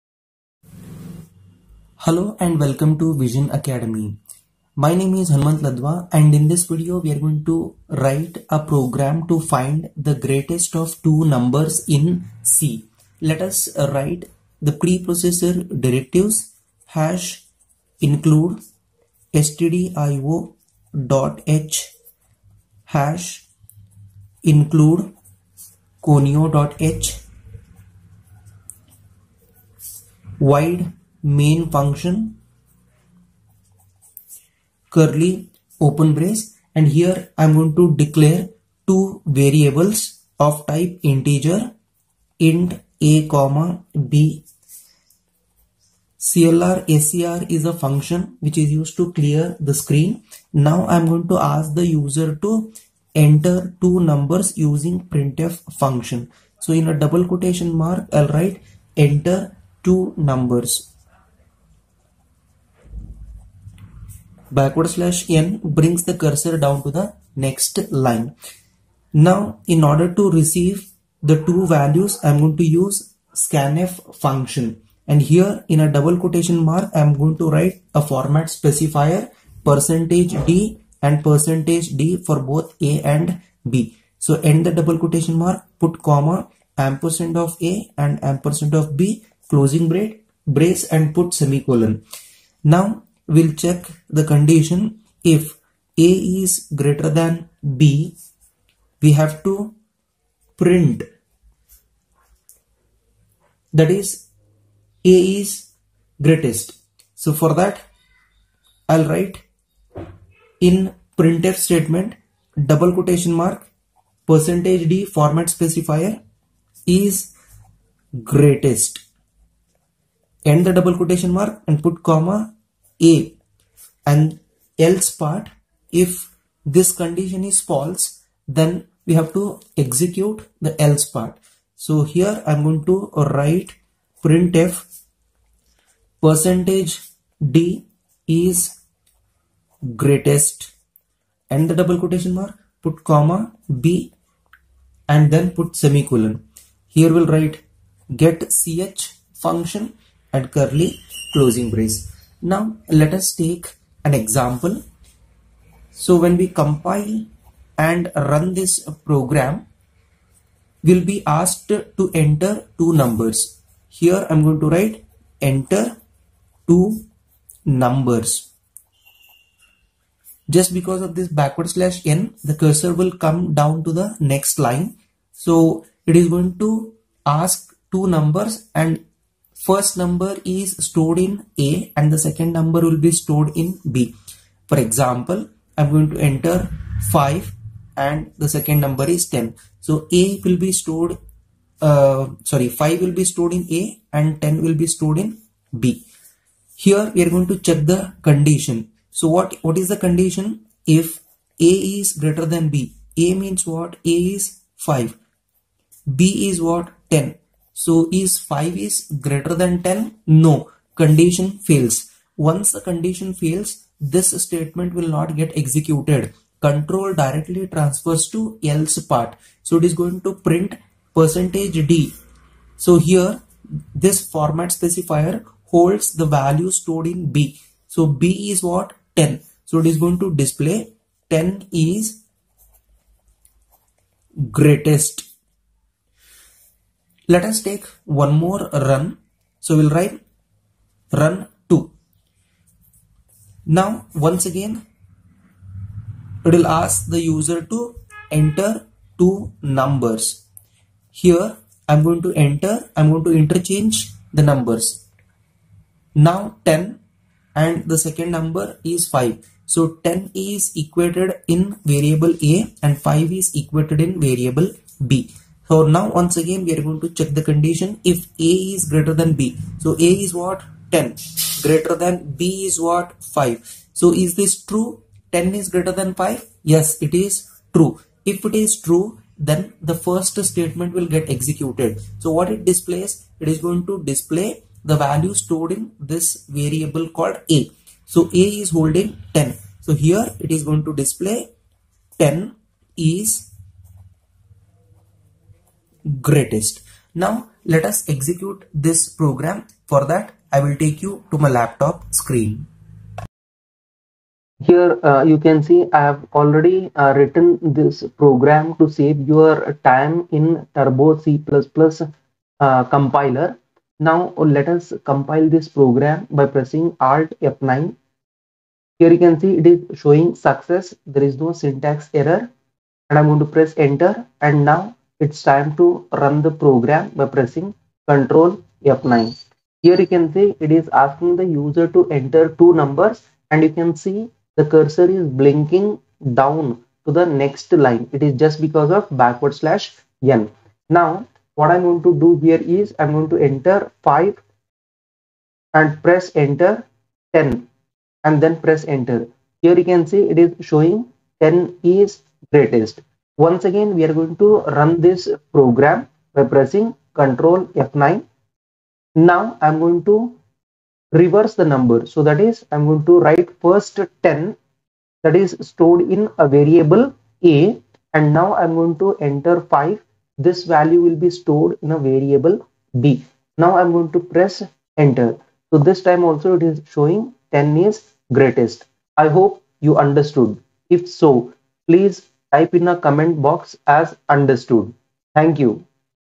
Write a program to find the greatest of two numbers in c using if else statement
In this programming in c tutorial video I have taught you how to Write a program to find the greatest of two numbers in c. I Hope you will enjoy it.

c language tutorial for beginners